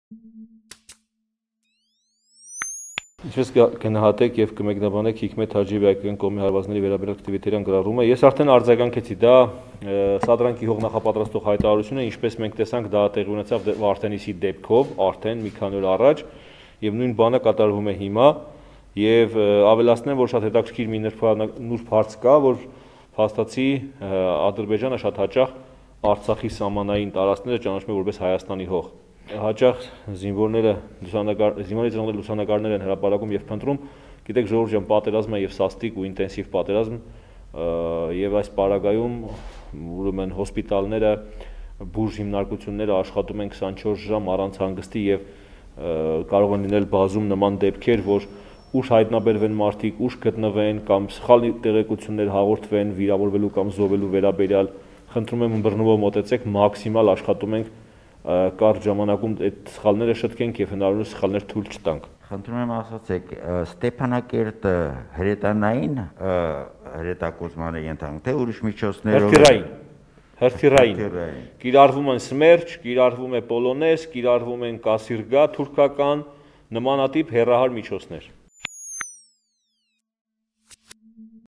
Ինչ հրթիռներով են կրակում Ստեփանակերտի վրա. Ներկայացնում է Արծրուն Հովհաննիսյանը
Սաստիկ պատերազմ է․ կարող են լինել դեպքեր, երբ զինվորներն ուշ հայտնաբերվեն, կամ սխալ հաղորդվի զոհվելու մասին. ՊՆ